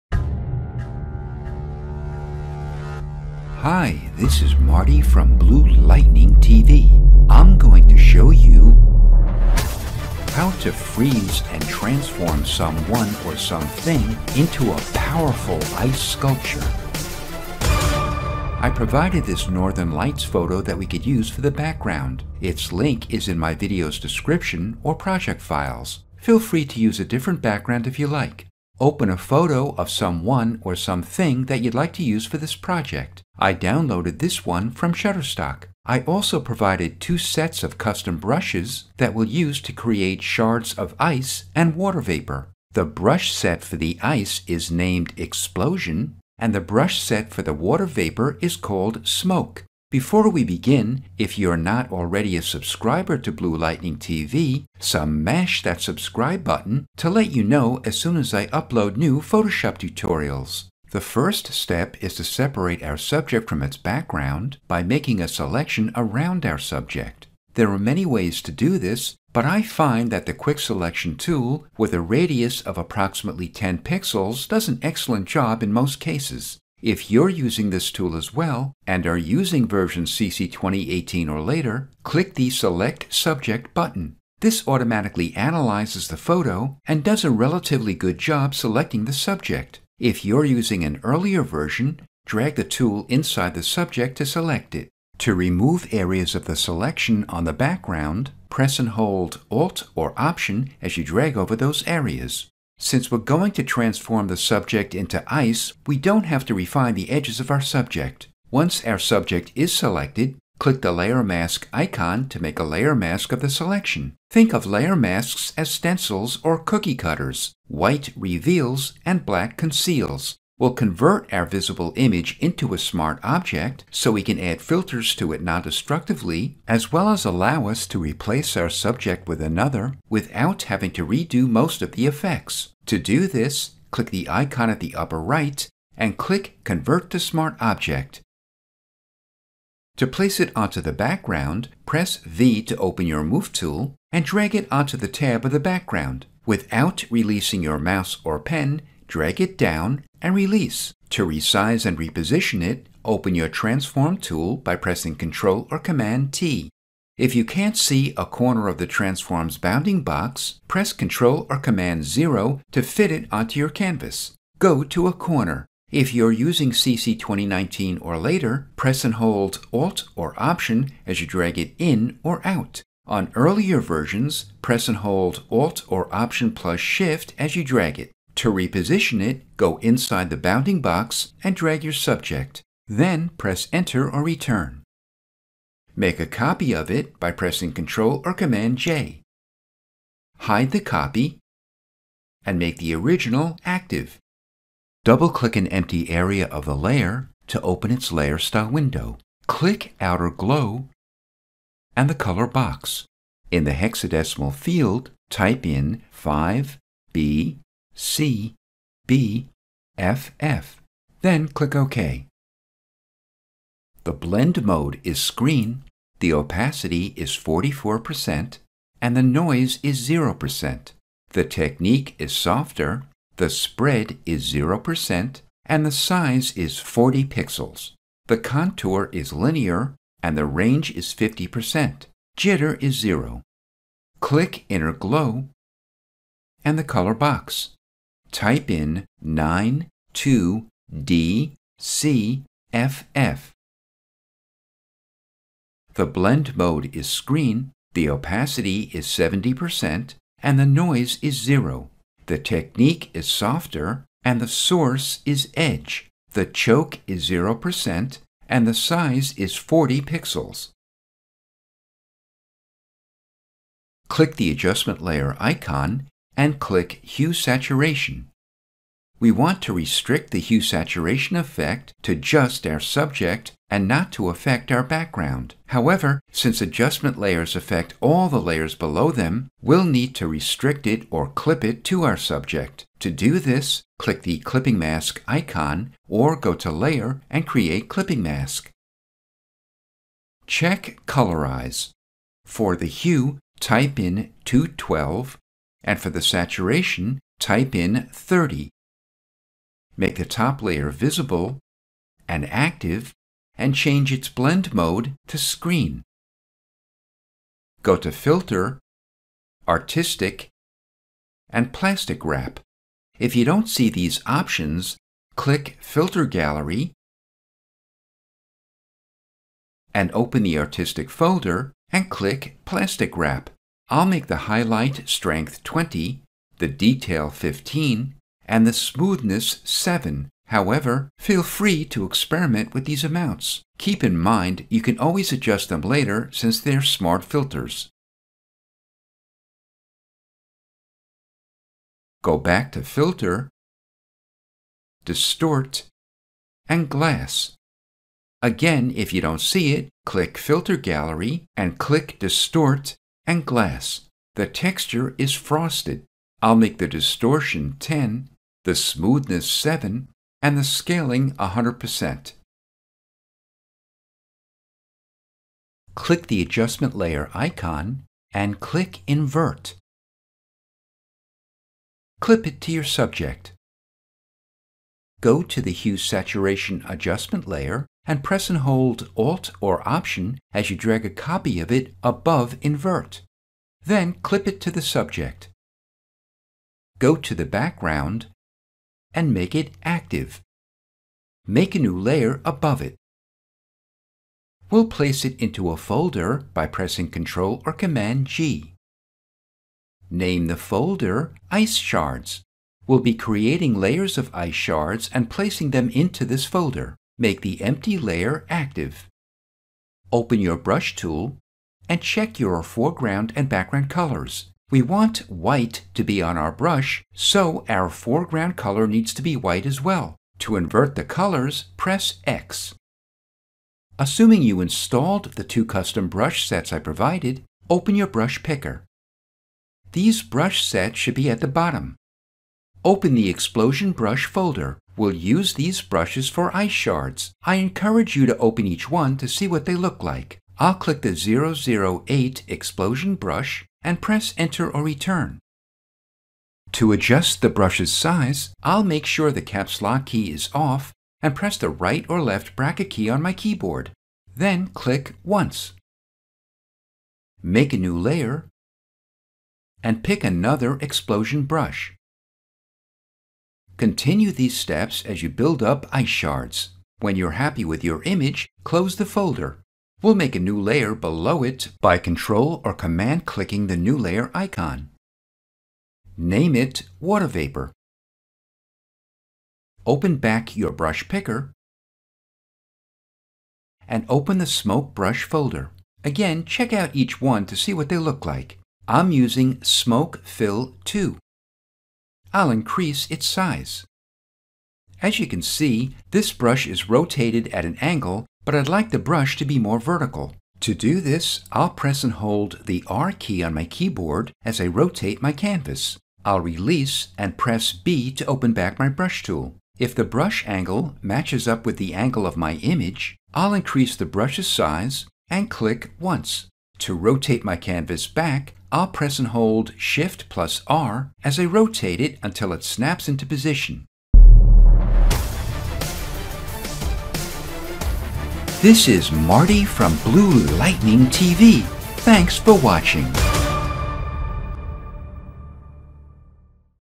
Photoshop: Create an ICE SCULPTURE of Someone!
Photoshop CC 2021 tutorial showing how to flash-freeze someone or something into an ice sculpture.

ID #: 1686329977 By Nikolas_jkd

BLTV MERCH @ Redbubble:  BLTV.redbubble.com